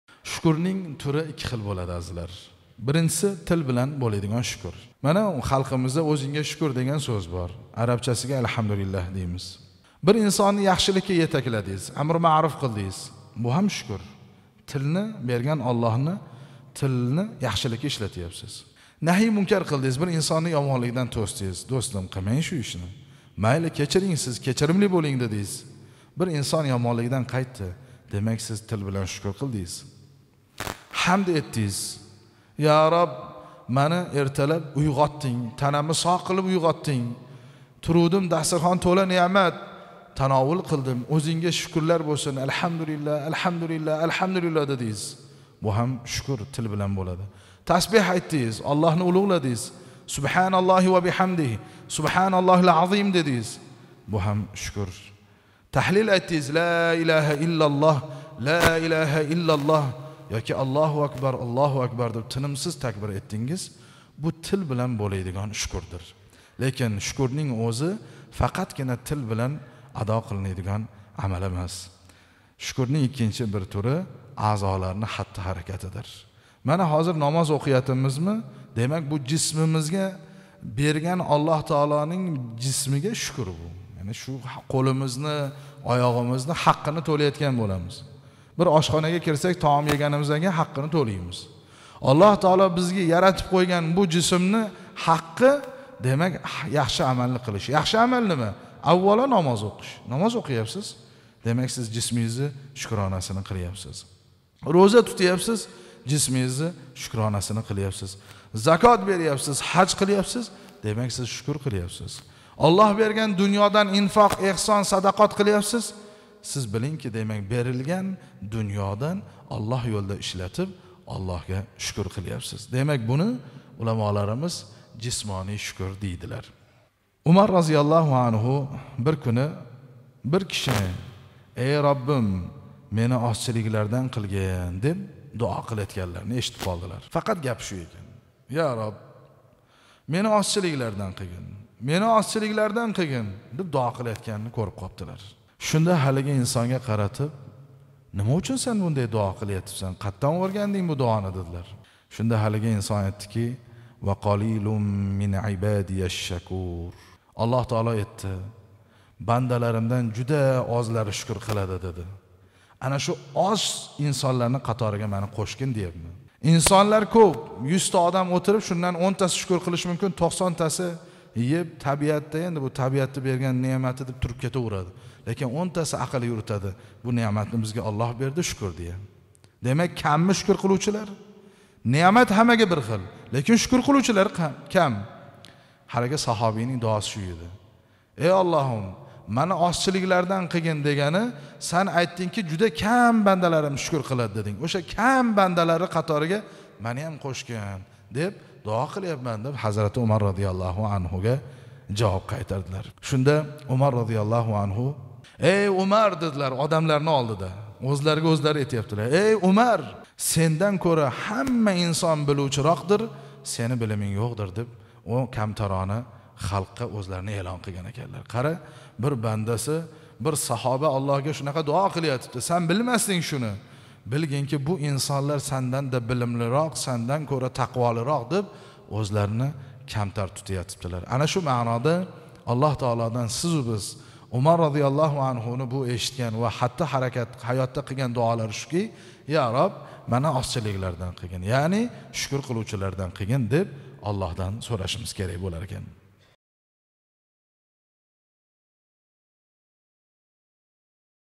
Шукурнинг икки ҳили
“Шайх Зайниддин” жоме масжиди имом хатиби
Абдураҳманов Яҳё Убайдуллоҳ ўғли